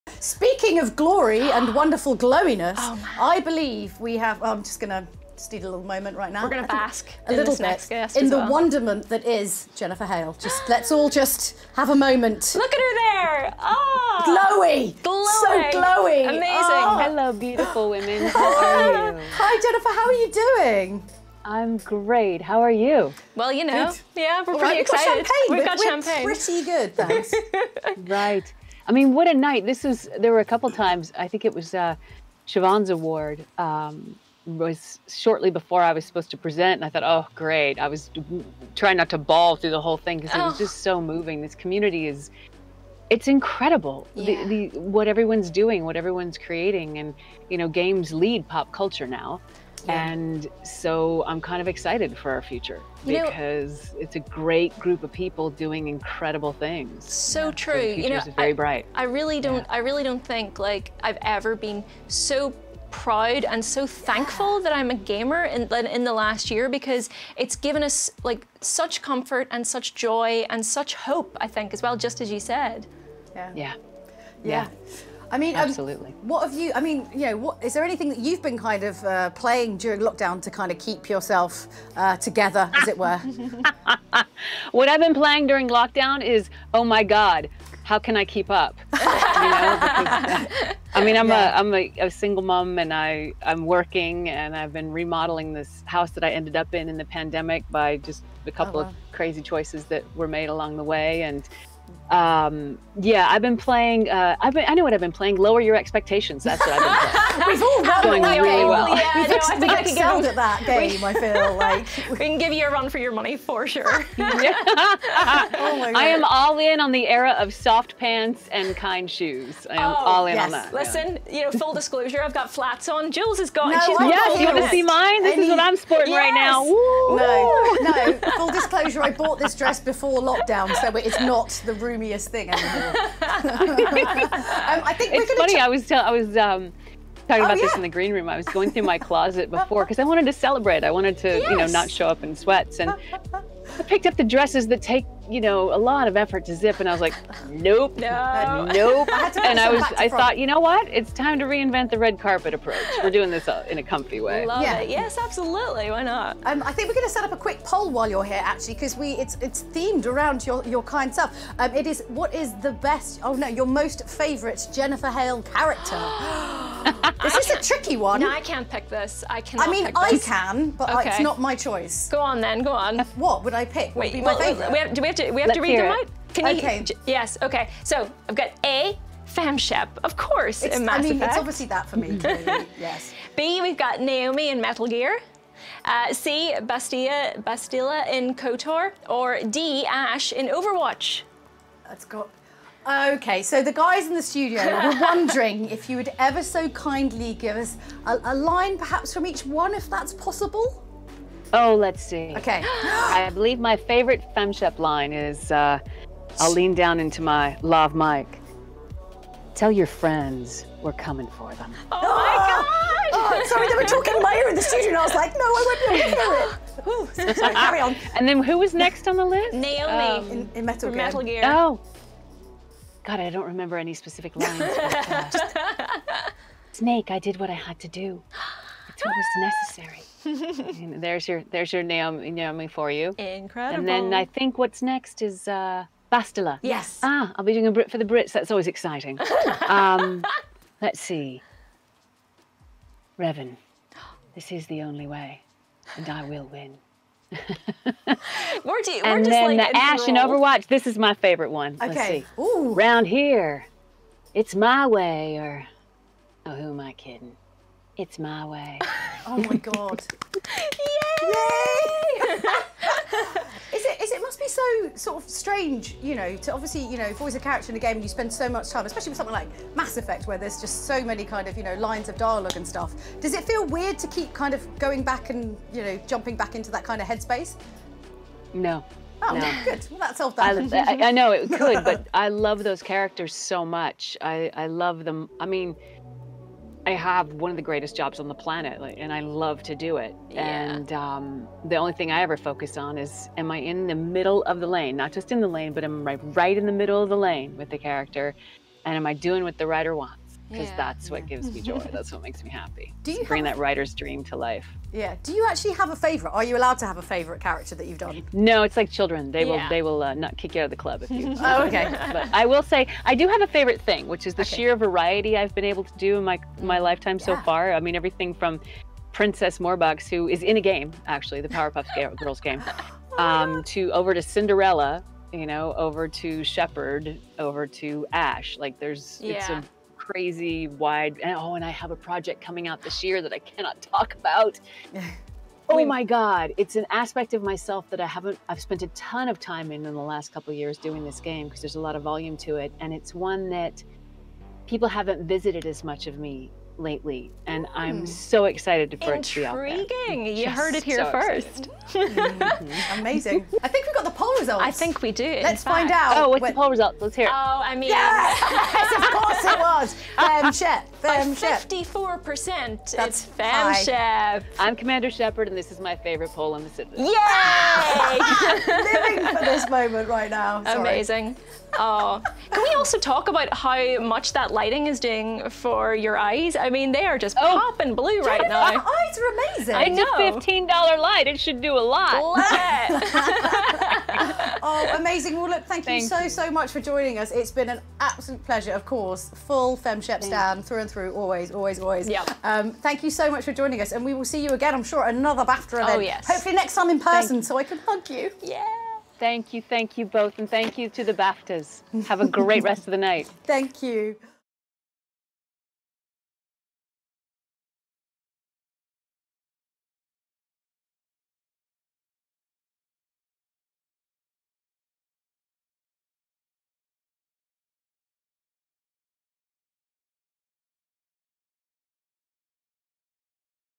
Jennifer Hale Shows Off Some Famous Lines from Her Voice Acting Career | BAFTA Games Awards 2021
Jennifer Hale discusses staying positive over the last year and her iconic roles during her voice acting career.